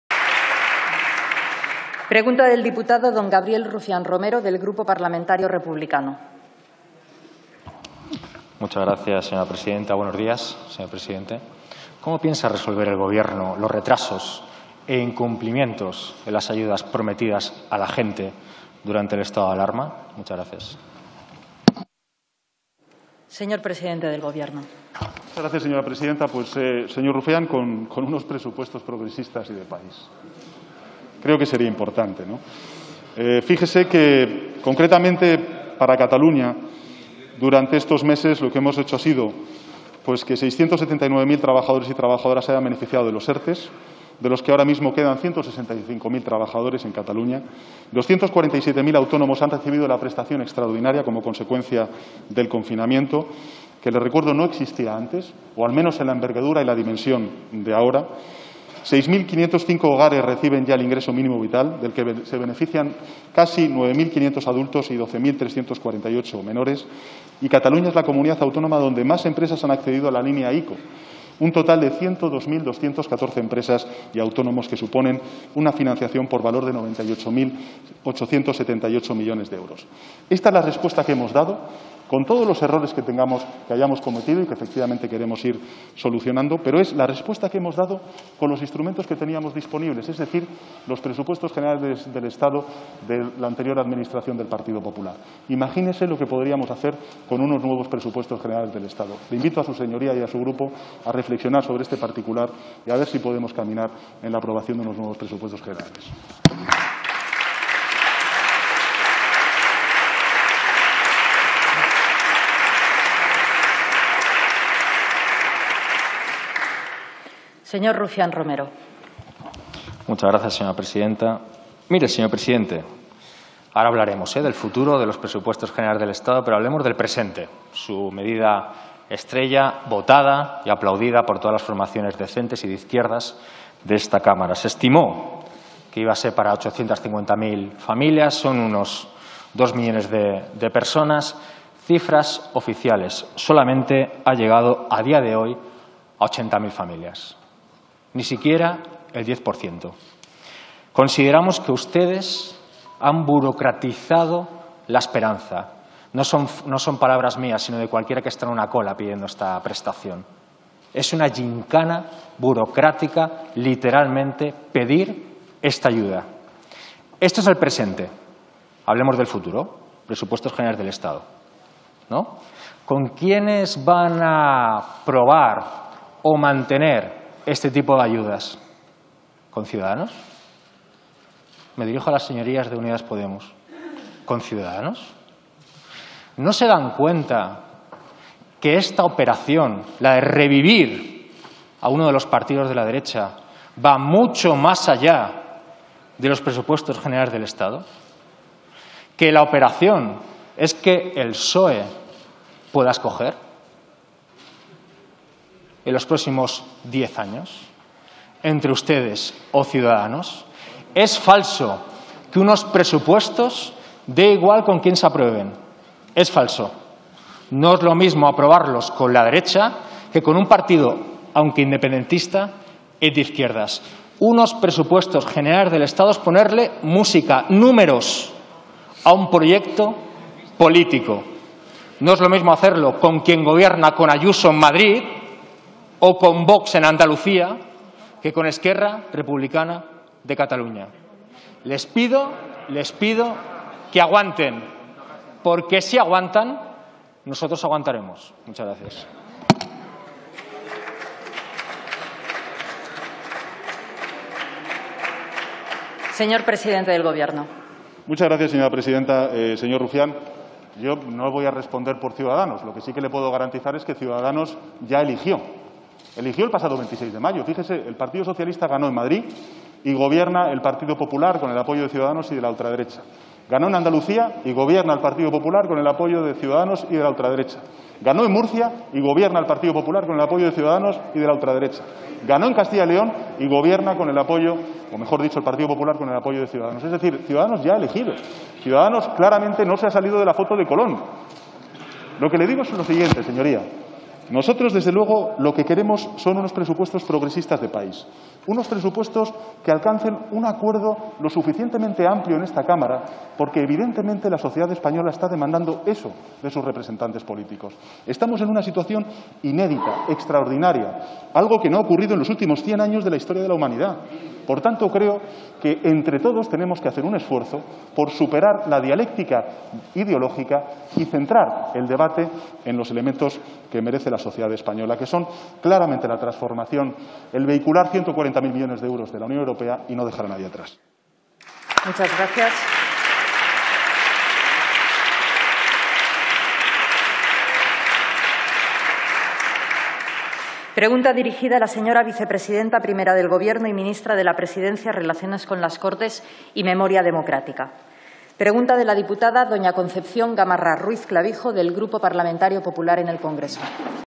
Pregunta de Gabriel Rufián a Pedro Sánchez hoy 9 de Septiembre de 2020 en el pleno del Congreso
¿Cómo piensa el Gobierno resolver los incumplimientos en las ayudas anunciadas durante la pandemia?